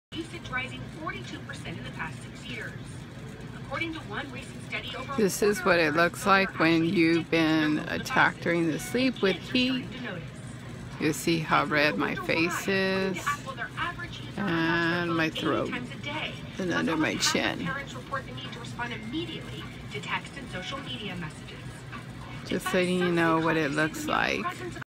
03/26/17 my face and neck from exposure to frequencies
Just a quick video of a face attack.   My face didn't look like that b4 I want to 😴 sleep.